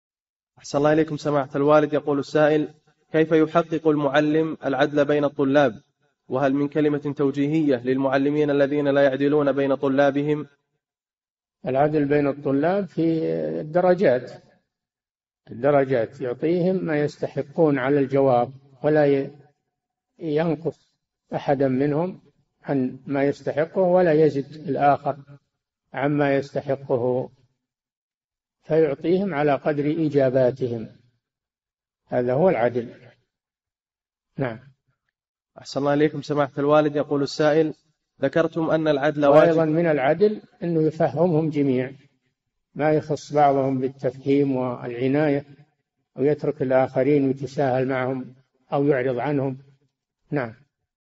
[770- 4717] كيف يكون عدل المعلم بين طلابه؟ - الشيخ صالح الفوزان
"[4717-770] كيف يحقق المعلم العدل بين الطلاب؟ وهل من كلمة توجيهية للمعلمين الذين لا يعدلون بين طلابهم؟
الجَوَابُ: العدل بين الطلاب في الدرجات، الدرجات يعطيهم ما يستحقون على ""الجَوَابُ، ولا ينقص أحدًا منهم عما يستحقه ولا يزد الآخر عما يستحقه، فيعطيهم على قدر إجاباتهم، هذا هو العدل، وأيضًا من العدل أنه يفهمهم جميعًا ما يخص بعضهم بالتفهيم والعناية ويترك الآخرين ويتساهل معهم أو يعرض عنهم.""
فتاوى المجالس في تفسير المفصل
للاستفسار والإقتراحات والدعم
رئيس الفريق
لمشروع كبار العلماء